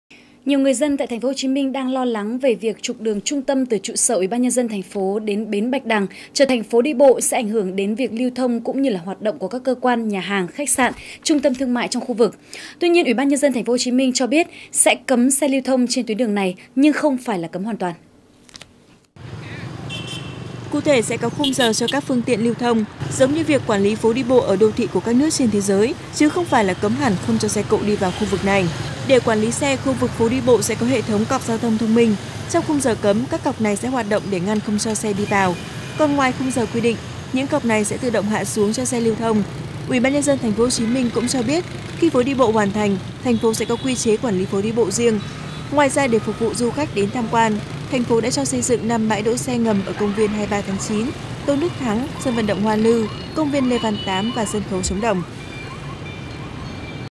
VTC14_TP HCM: Xe được chạy vào phố Nguyễn Huệ theo khung giờ
(Truyền hình VTC14) - TP HCM: Xe được chạy vào phố Nguyễn Huệ theo khung giờ:
Nhiều người dân tại TP HCM đang lo lắng về việc trục đường trung tâm từ trụ sở UBND thành phố đến Bến Bạch Đằng trở thành phố đi bộ sẽ ảnh hưởng đến việc lưu thông cũng như hoạt động của các cơ quan, nhà hàng, khách sạn, trung tâm thương mại trong khu vực. Tuy nhiên, UBND TP HCM cho biết, sẽ cấm xe lưu thông trên tuyến đường này nhưng không phải là cấm hoàn toàn.